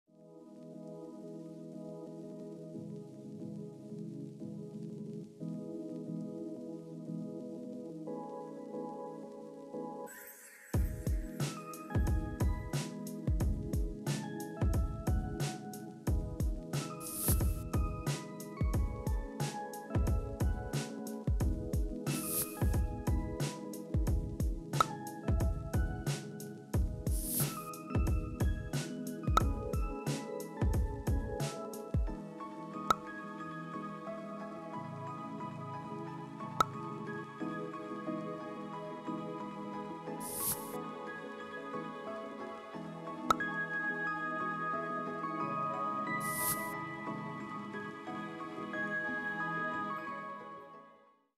Pokemon TCG Showcase and Review of Cara Liss Vivid Voltage Nr. 149 of 185
Hi, and welcome to my review of the Trainer Pokemon card of Cara Liss from the Vivid Voltage set!